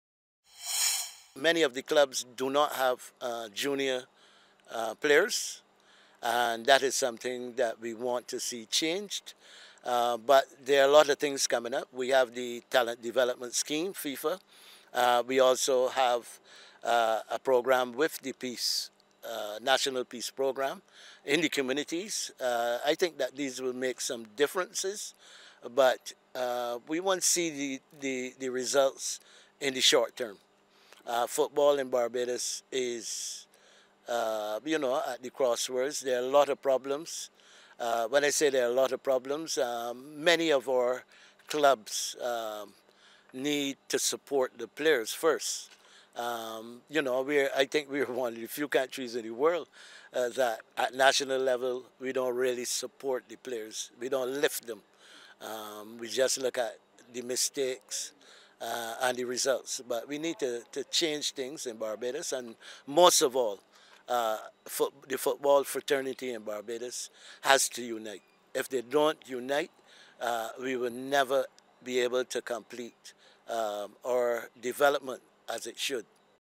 Local football at a 'crossroad'
Barbados Football Association President Randy Harris says the local governing body is committed to the development of players and clubs on the island, but needs the support of administrators and coaches in order to reap success regionally and internationally.

Harris was speaking during an interview after he was re-elected as president following the annual general meeting and congress.